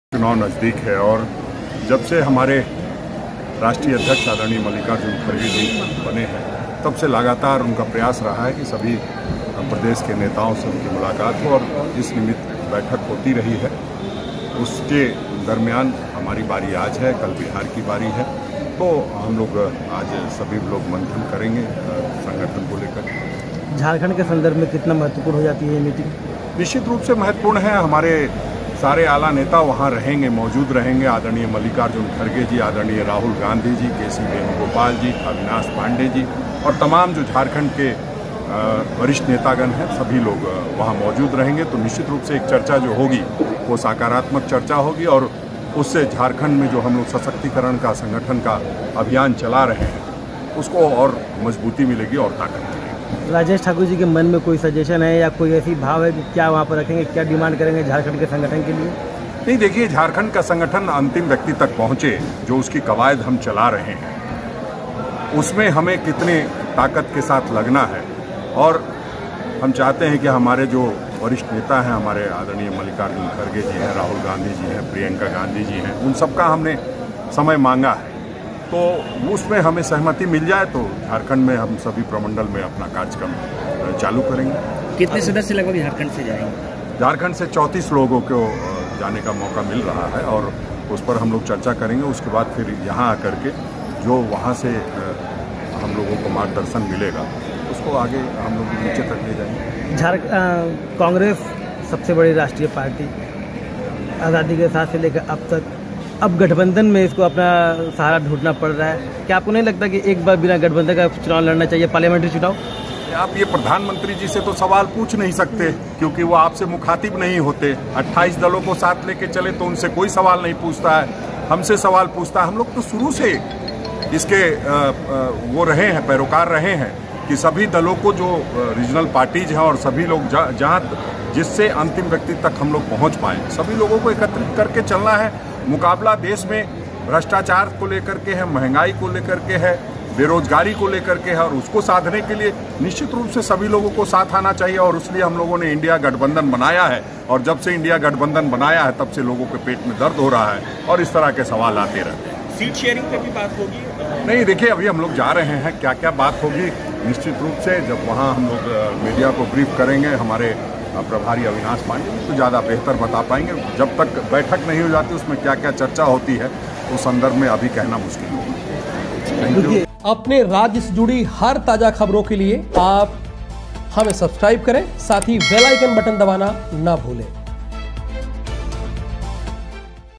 2024 लोकसभा चुनाव की सरगर्मी हुई तेज, कांग्रेसी नेता दिल्ली रवाना, सुनिए राजेश ठाकुर ने क्या कहा
2024 लोकसभा चुनाव की सरगर्मी हुई तेज, कांग्रेसी नेता दिल्ली रवाना, सुनिए राजेश ठाकुर ने क्या कहा